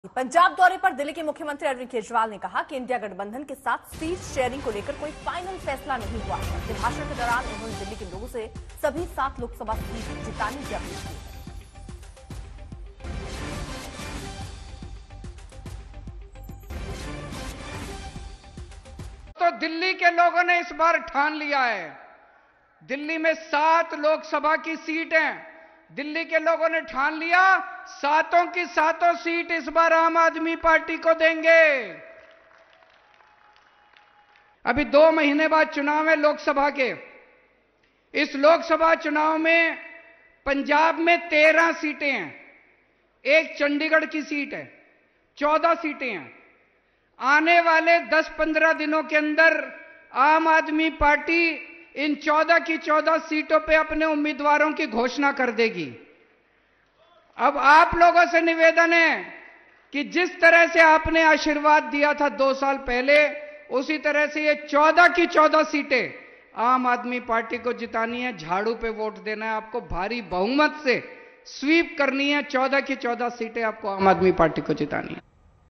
CM Kejriwal Speech Today: केजरीवाल ने Punjab के Tarn Taran में रैली के दौरान बड़ा बयान दिया | AAP
केजरीवाल ने पंजाब के तरनतारन में रैली के दौरान बड़ा बयान दिया है. केजरीवाल ने कहा कि दिल्ली के लोगों ने इस बार ठान लिया है कि दिल्ली की सातों लोकसभा सीटें इस बार आम आदमी पार्टी को देंगे.